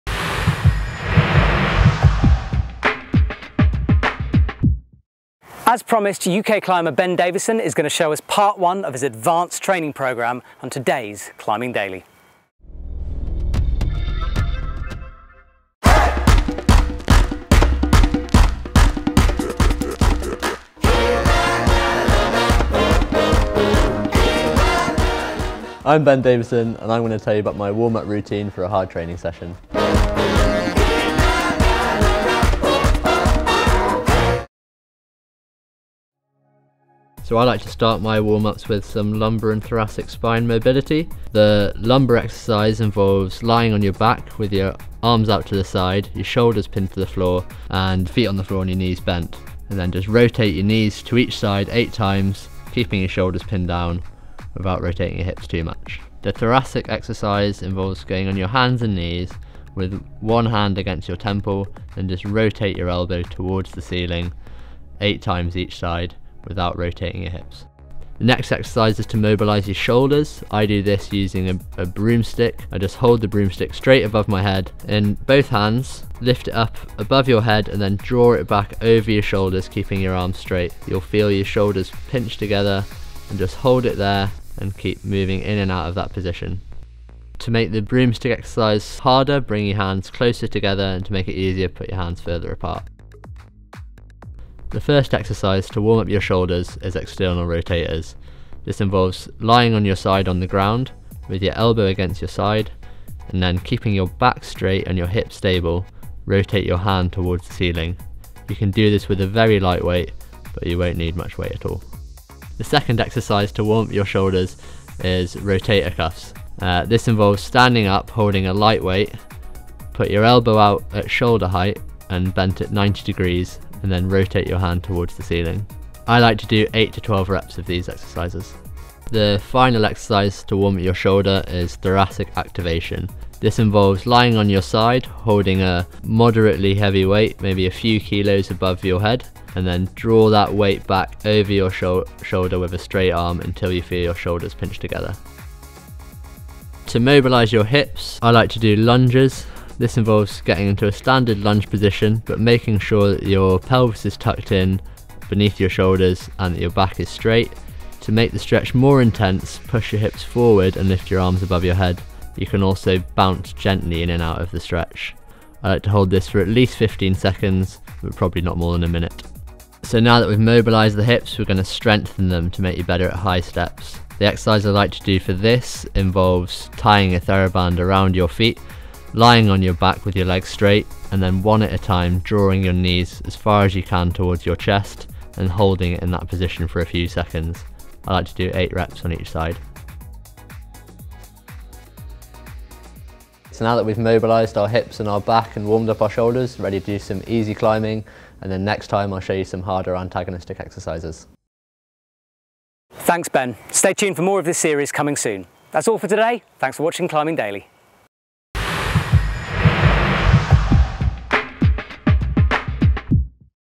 Ben Davison Advanced Training Programme: Warm Ups | Climbing Daily Ep.736
As promised, UK climber Ben Davison is going to be showing us part one of his advanced training programme on today's Climbing Daily.

First, we're taking a look at warming up before your session, with some interesting stretches to help prevent injury.

Remember, this is training aimed at climbers who want to start pushing their grade. The exercises are tough, so do not attempt them if injured. Listen to your body and don’t train to the point of injury.

Ben Davison’s Return From A Ground Fall In Flatanger | Climbing Daily Ep.735